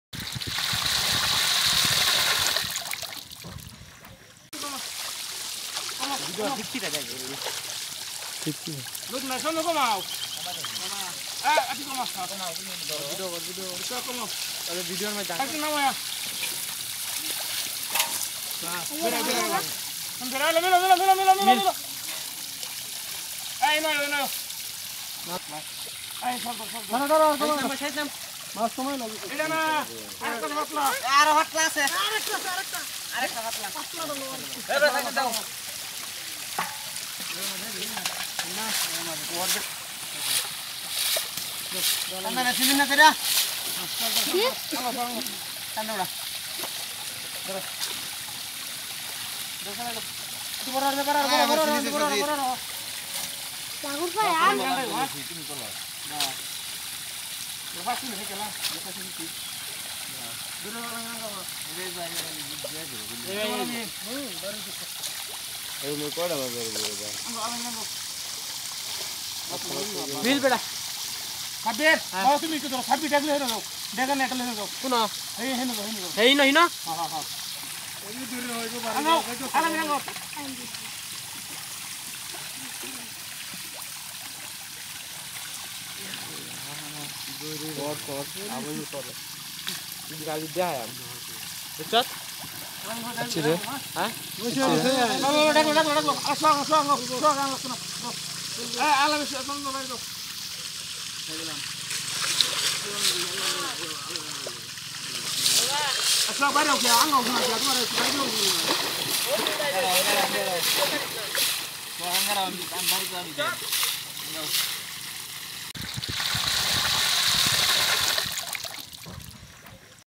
million little baby fish in the pond
Welcome beck to my YouTube channel.
Guys our team now showing  you the million baby fish product ..this fish are framd by our big fram and careing by a gentle team..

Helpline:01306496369

🖤♥️This account fish and Cooking official 🖤♥️